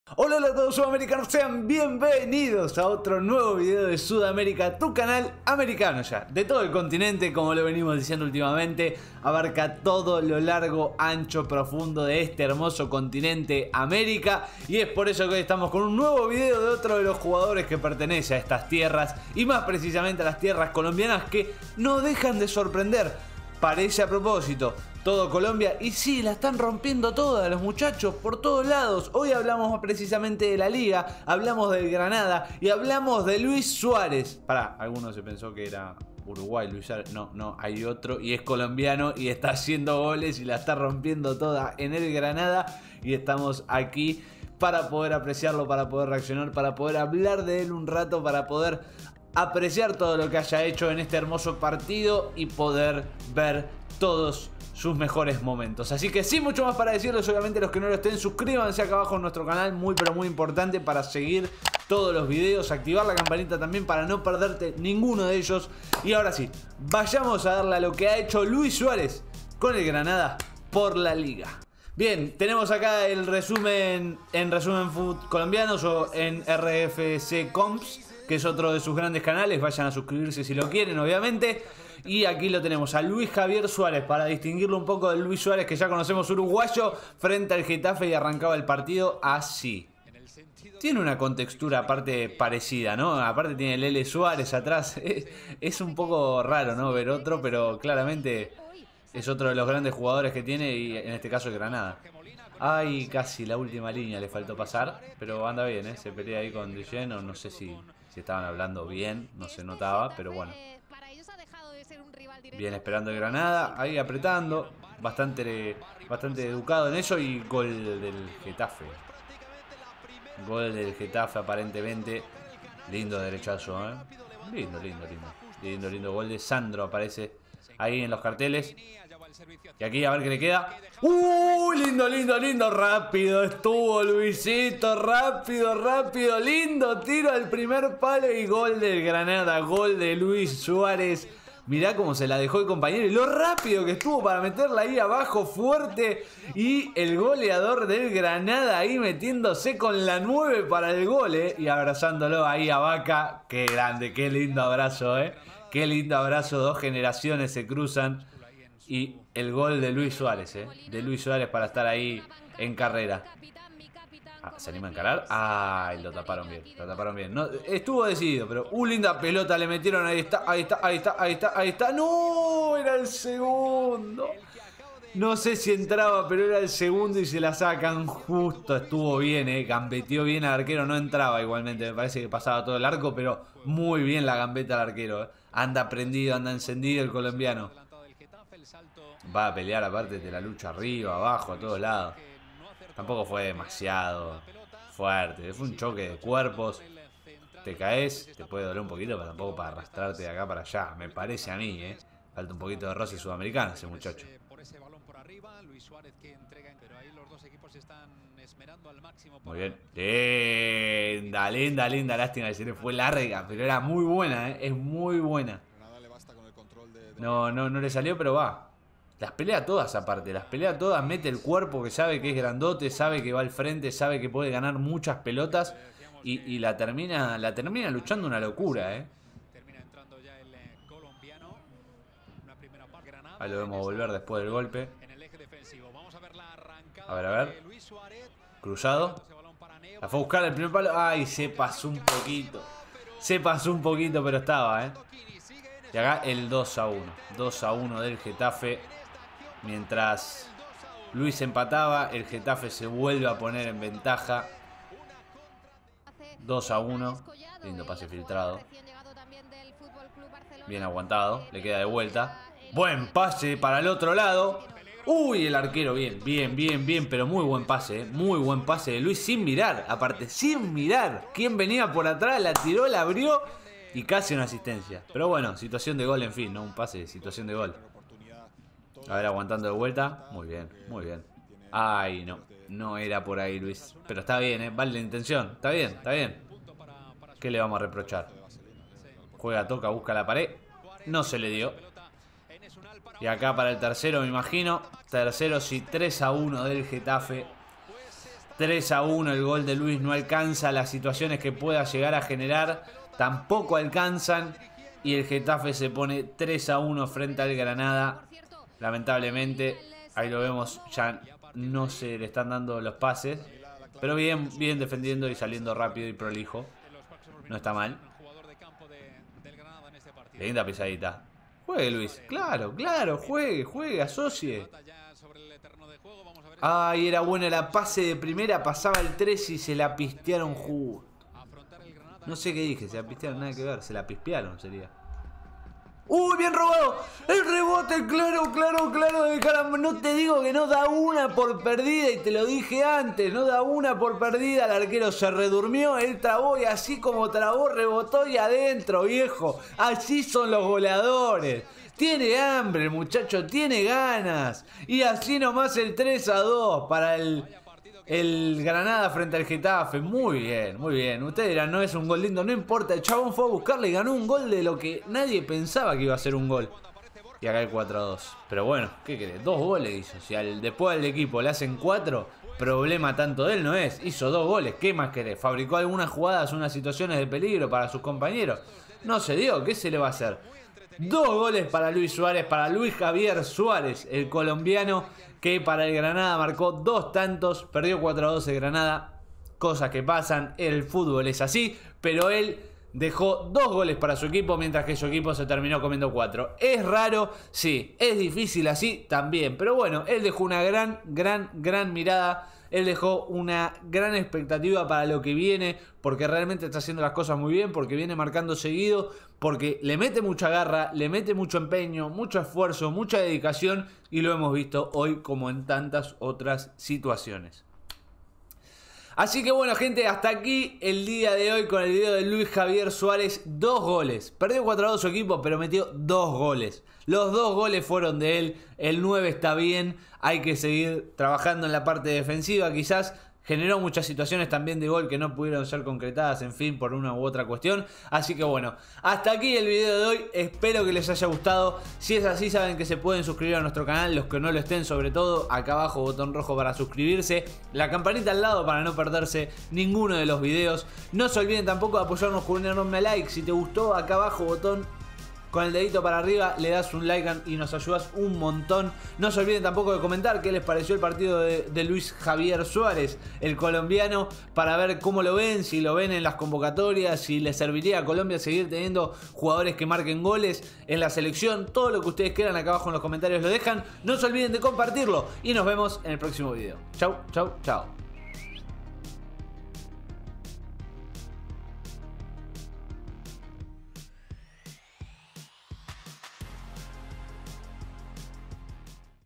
🇦🇷😱 ARGENTINO REACCIONA a 🇨🇴 LUIS SUAREZ vs GETAFE 🇪🇸 LA LIGA *HACE GOLES PERO NO ALCANZA*
LUIS JAVIER SUÁREZ es un DELANTERO COLOMBIANO que milita para el GRANADA FC. El está haciendo goles pero su equipo muchas veces pierde de todas maneras por no poder aguantar en su portería...